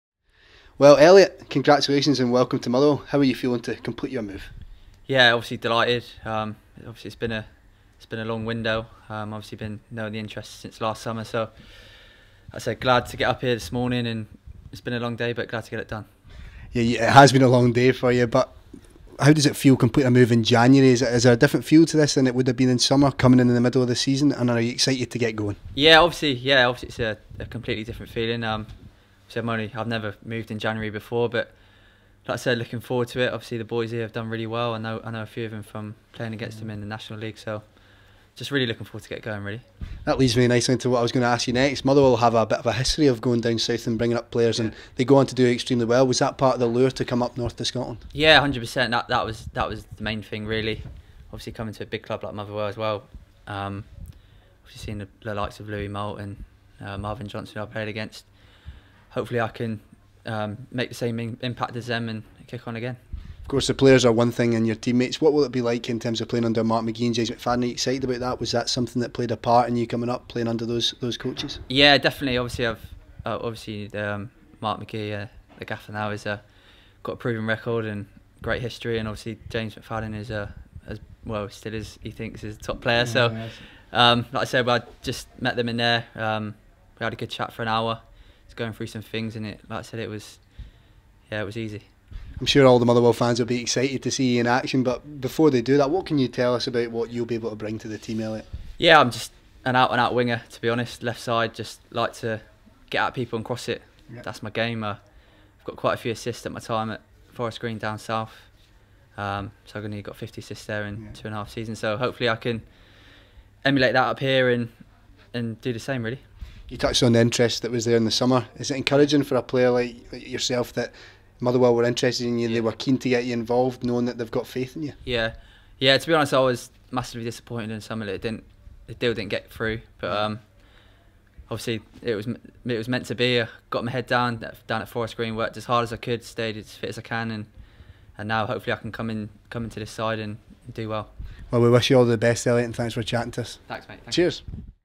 Elliott Frear signs for the Steelmen
Elliott Frear chats to MFC TV after completing his switch from Forest Green Rovers - penning a two-and-a-half year deal. Welcome Elliott!